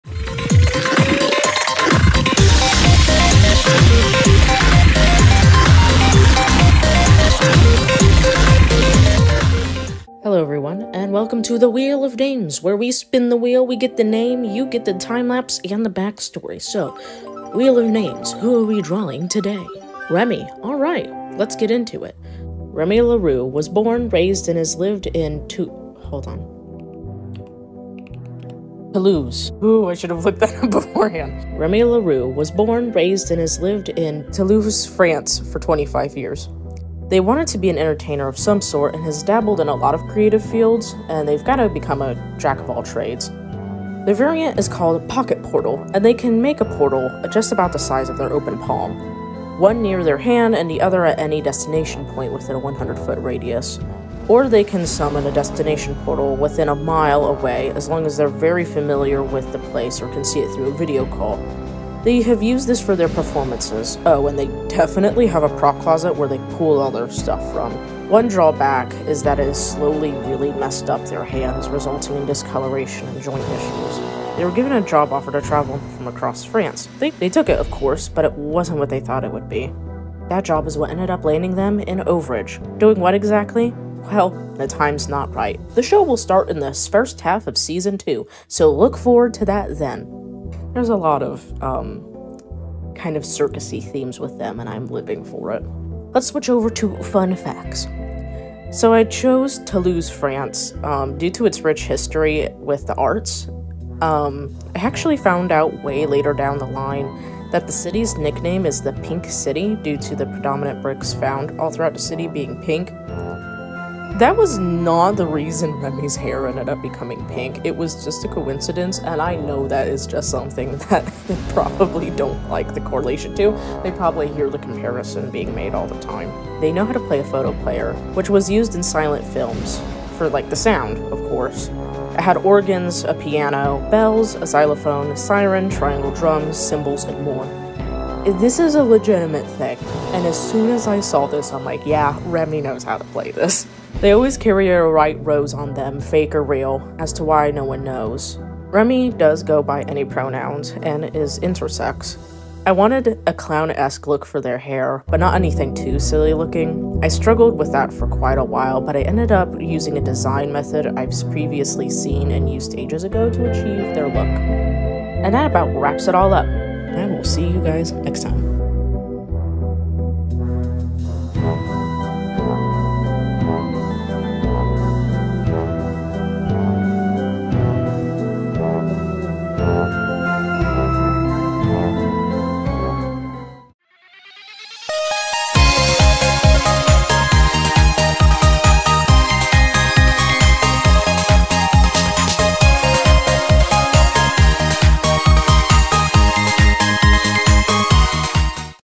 Wheel of Names! #2: Remi LaRue
There’s not too much currently known about Remi. Odd, considering they are supposed to be a well-known performer. Maybe they are just to themselves, or something else is going on…? Just have to wait and see.

Got any questions or comments? Have a good week!

Wkh gdwd lv rq glvnhwwhv? L grq'w fduh li zh grq'w kdyh d zdb wr uhdg wkhp uljkw, qrz, zh zloo ilqg d zdb. Qr, brx duh qrw wr vwhdo dqbwklqj hovh, L grqw' fduh li wkhb kdyh d uhdghu- mxvw jr!